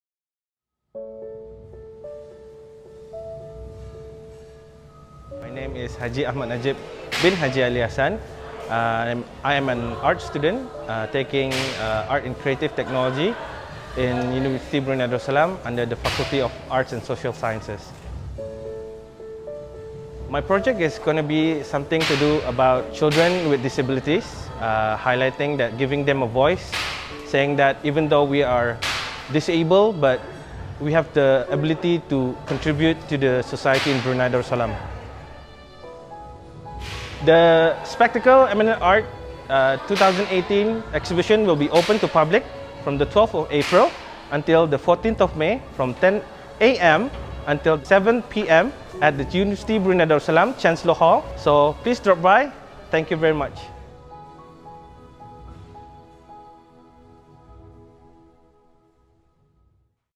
Spectacle 2018 | Eminent Art | Najib Ali - I'm the same but different
Najib's sculpture and mixed media installation tackle the subject of children with disabilities. By receiving the right education and treatment, Najib believes that these children can make valuable contributions to society.